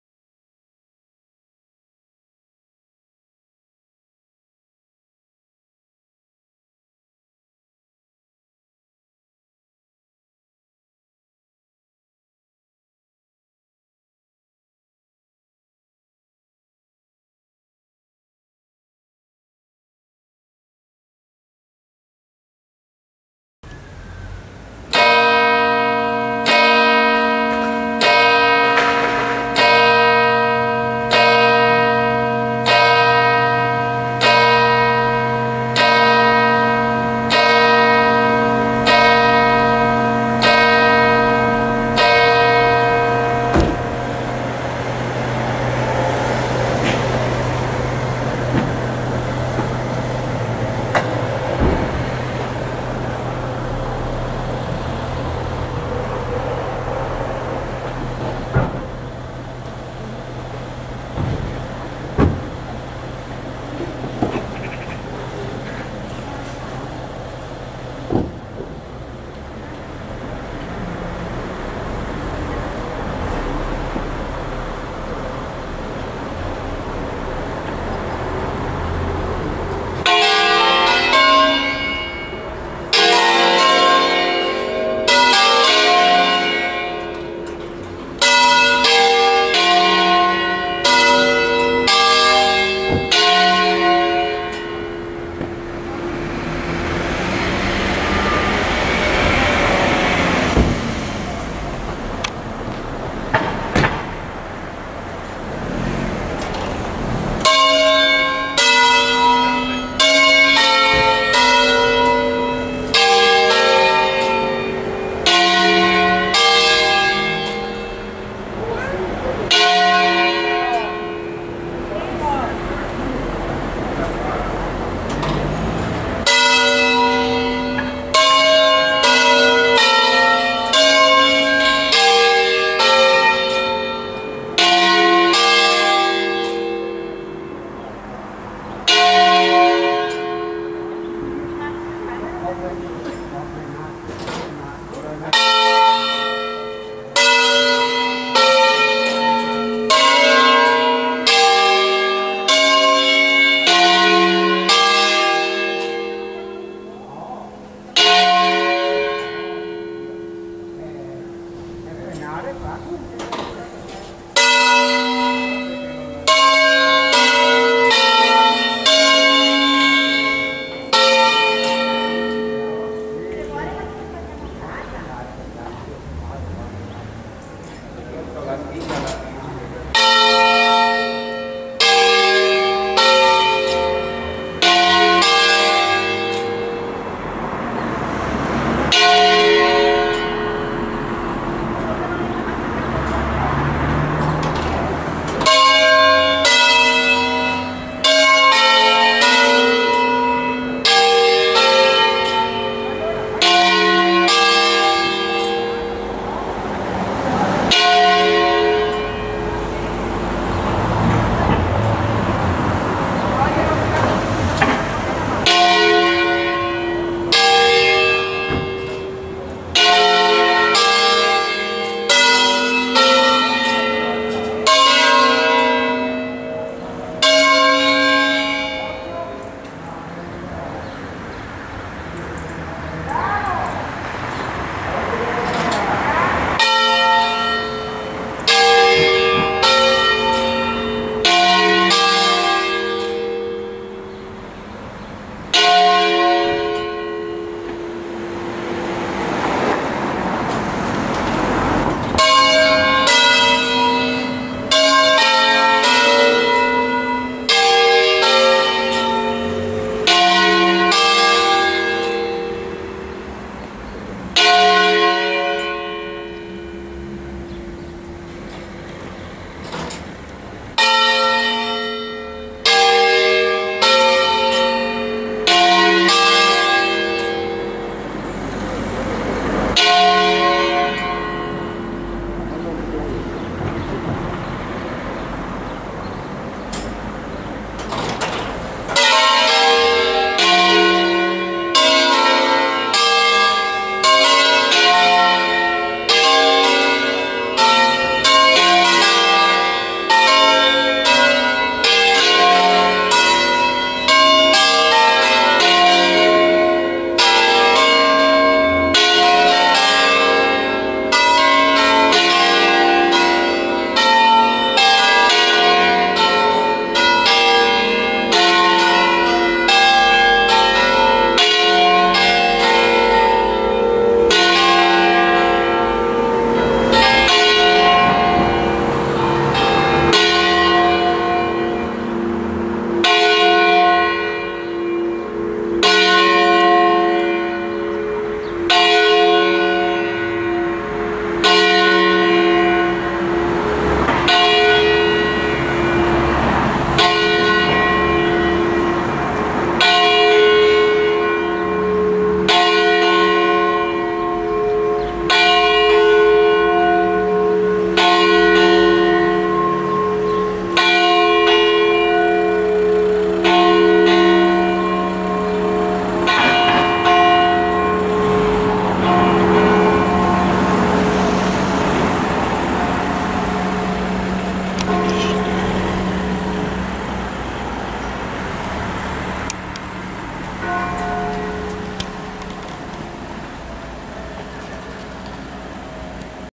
Le campane di Pontoglio (BS)
Chiesa Parrocchiale di S. Maria Assunta
Pontoglio (BS) - Bassa bresciana occidentale

Concerto da 9+1 campane in Si♭²
Paolo Capanni - Castelnovo nè Monti 1964

Concerto solenne delle 5 campane minori in Fa³ x l'Angelus nella solennità del Santo Natale. Ore 12

Buona visione